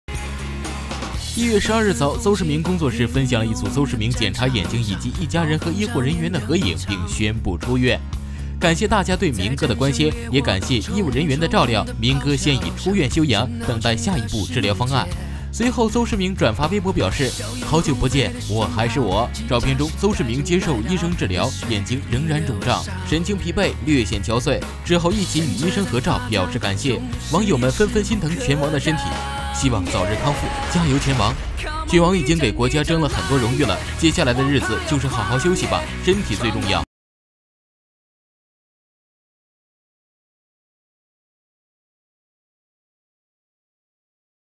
【2年前】鄒市明出院后首公開發聲：好久不見 我還是我 網友紛紛擔心拳王的身體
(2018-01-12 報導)

鄒市明出院后首公開發聲：好久不見 我還是我 照片中鄒市明接受醫生治療 眼睛腫脹 神情疲憊 略顯憔悴 網友紛紛擔心拳王的身體